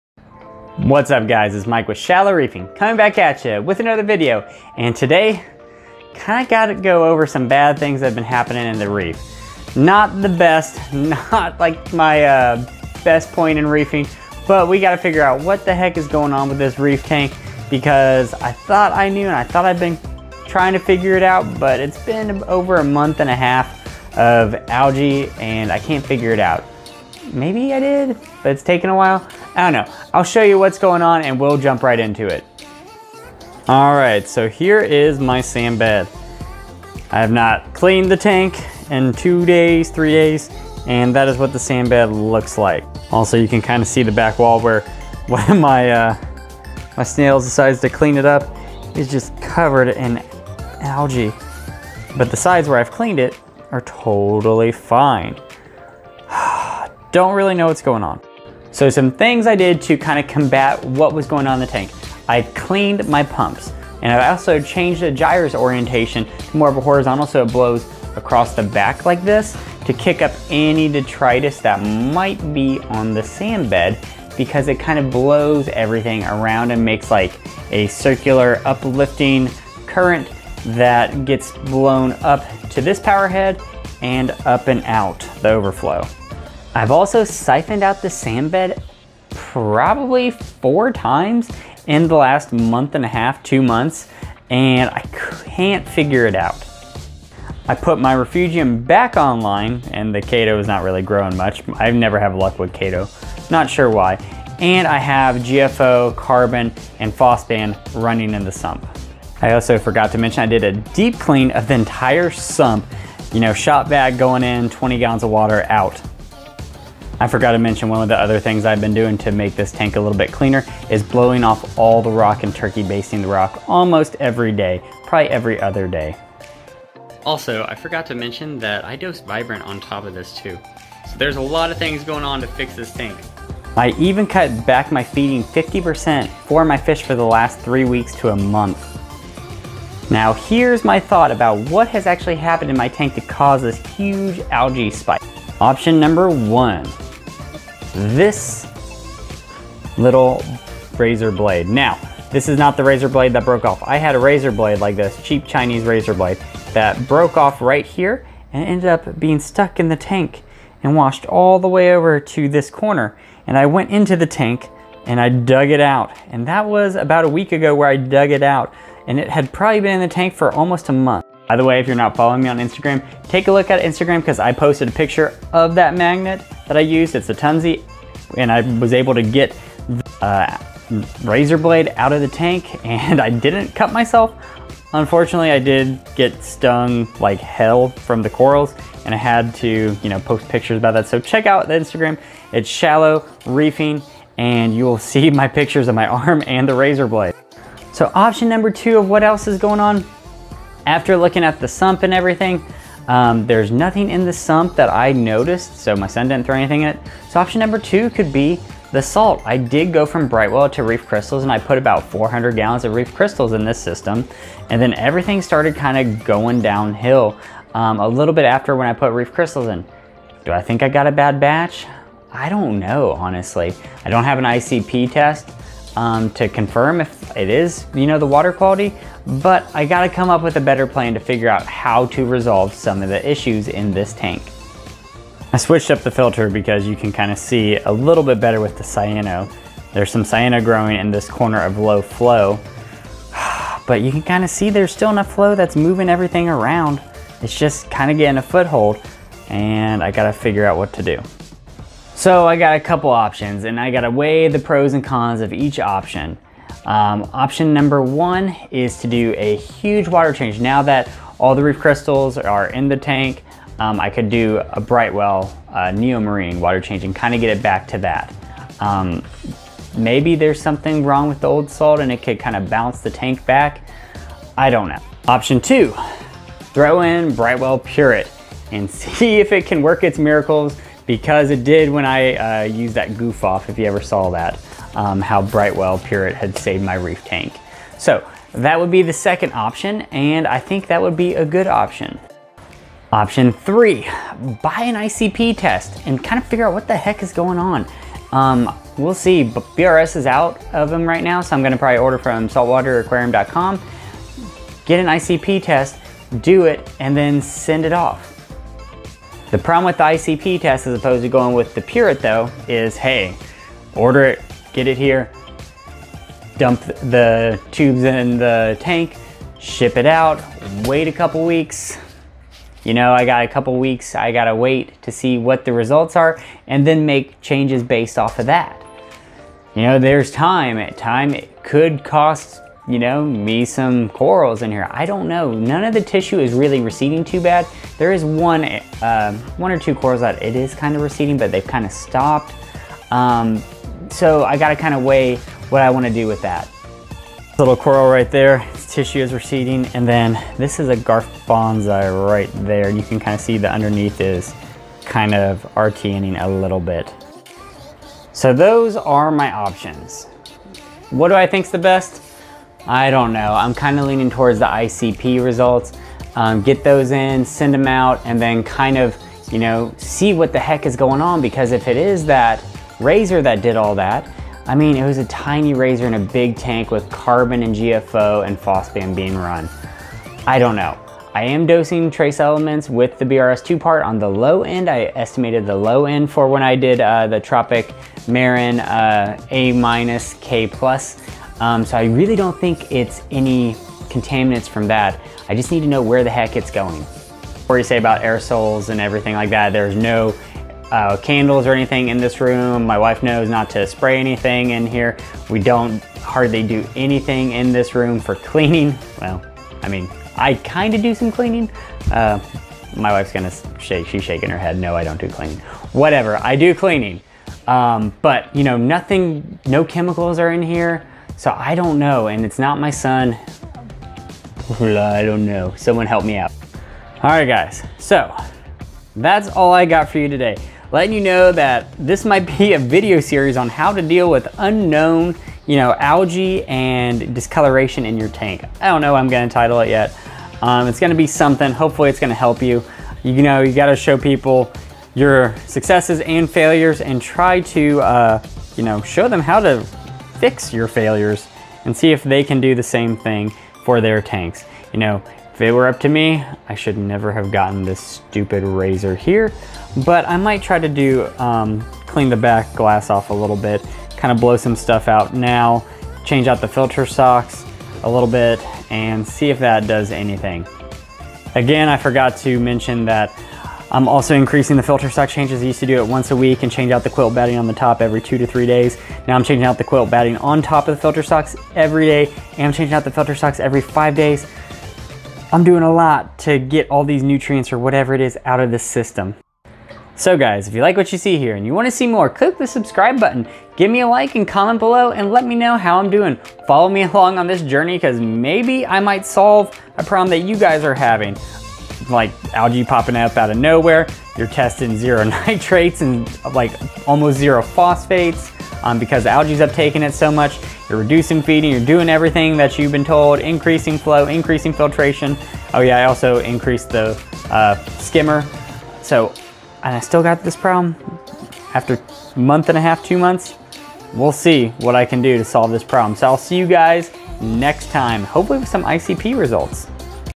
Solving Unknown Algae and Cyano Problems in my 200 Gallon Reef Tank- Part 1
Let's see if I can fix this algae and cyano issue. I have no idea where it came from. Maybe bad salt? Maybe a razor? Only an ICP test will tell me the truth.

Parameters
35.5 salinity. 8.3 alkalinity 430 calcium 1440 magnesium 0.0 nitrate and 0.06 phosphate.